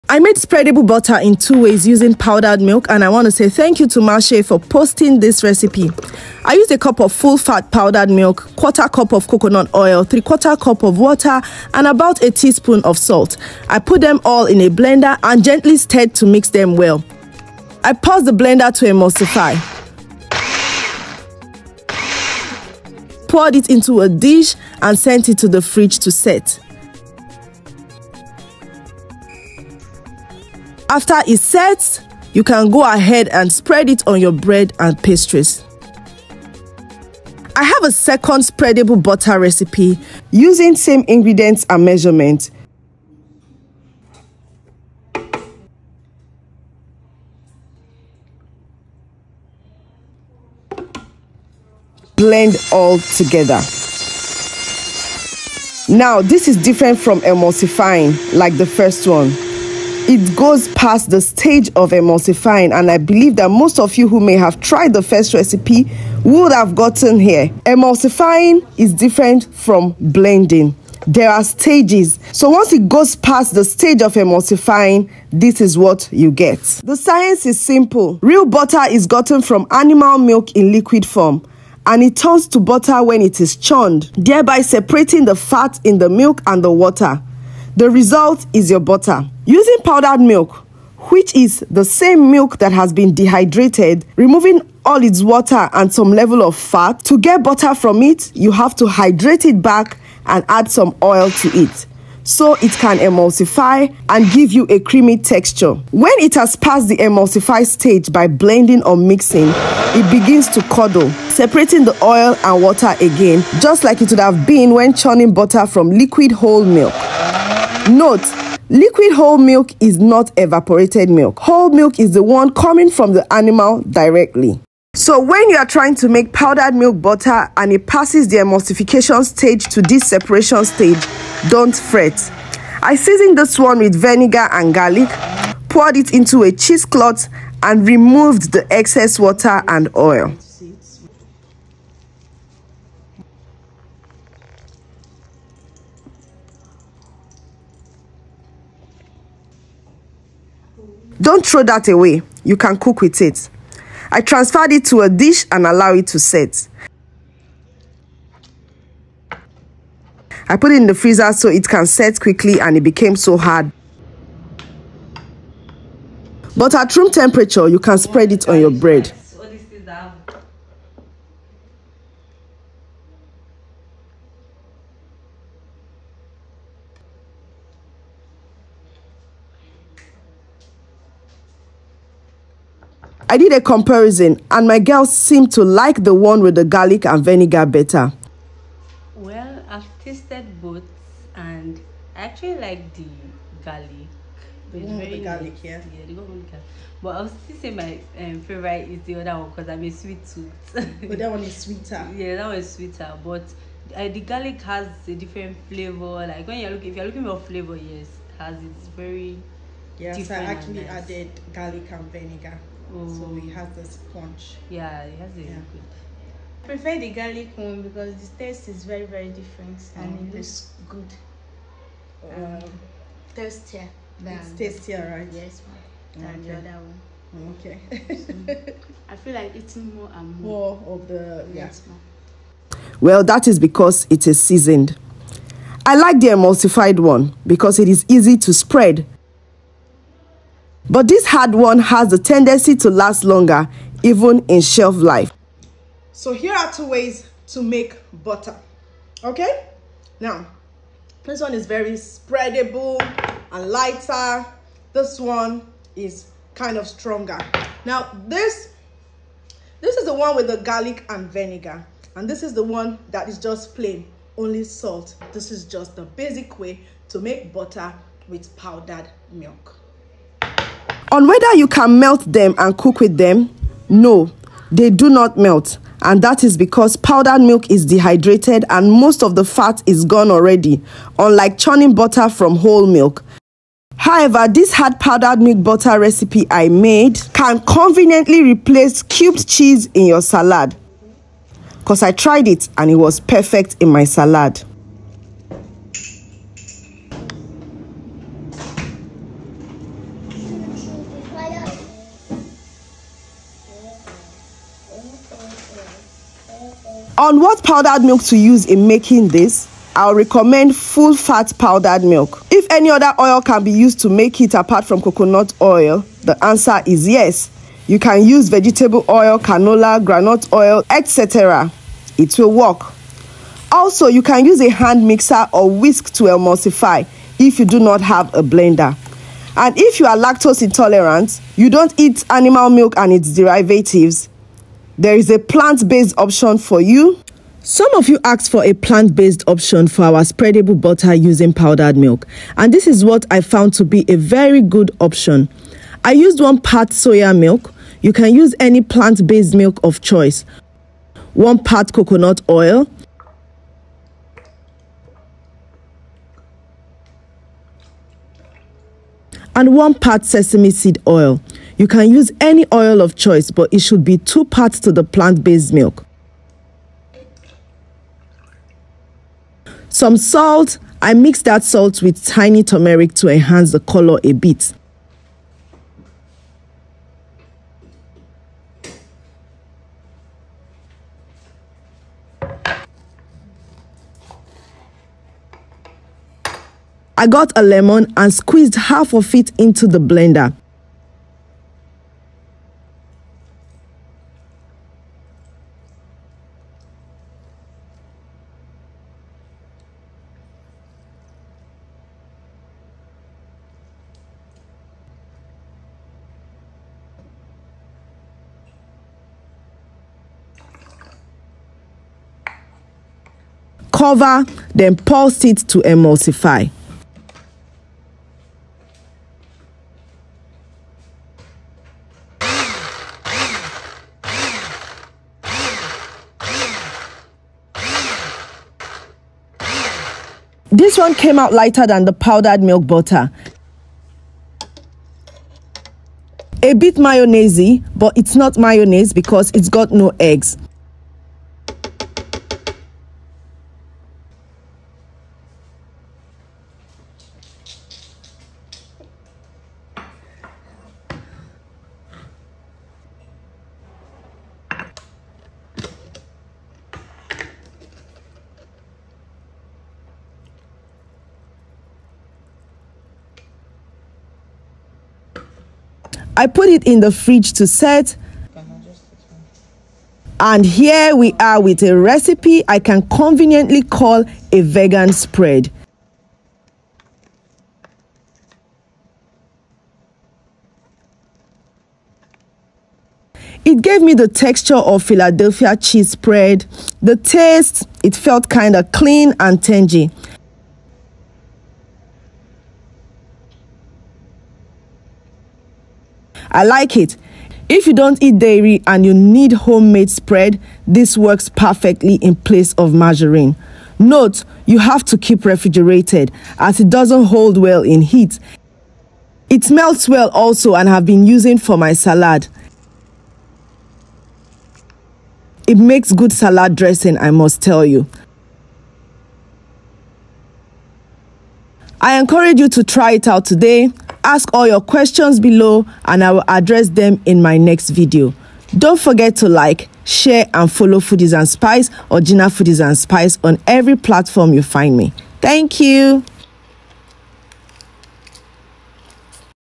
How to make Butter with Powdered Milk and Vegan Margarine
I made 2 types of butter using powdered milk and some of my followers asked for a vegan option. So try them. The ingredients are easy to get:

Ingredients:
- Full-fat powdered milk
- Coconut milk
- Salt
- Water
- Garlic (optional)

Vegan option:
- Soya milk (Or any other plant-based options)
- Coconut oil
- Sesame seed oil
- Salt
- Tumeric
- Lemon

To share your videos or experiences recreating our recipes, visit these social media handles: